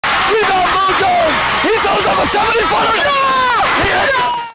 The Shot
Bo Jones hits a 70 ft shot to win the game vs. ECU